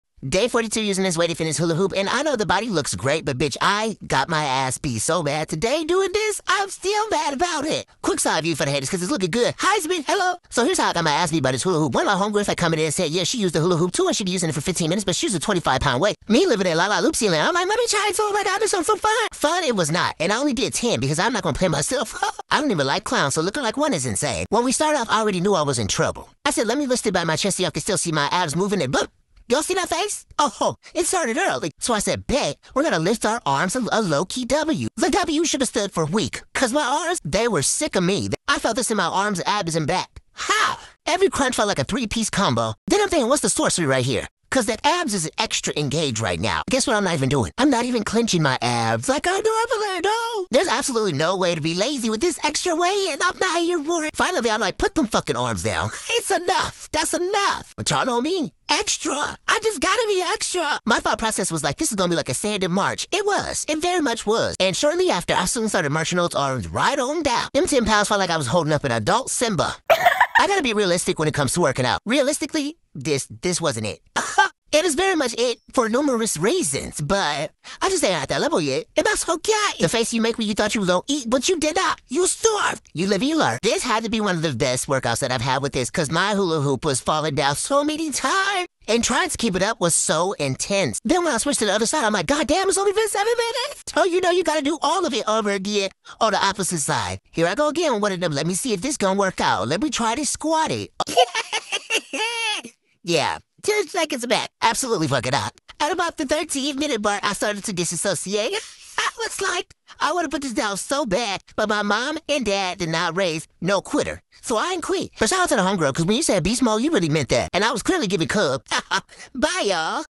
Day 42 using the weighted hula hoop | adding 10 pound weight
Hey pals! Day 42 using the weighted hula hoop. I had this crazy idea to try this 10 pound weight and the hula hoop. Let’s just say this was a greattttt workout!🤌🏽🙌🏾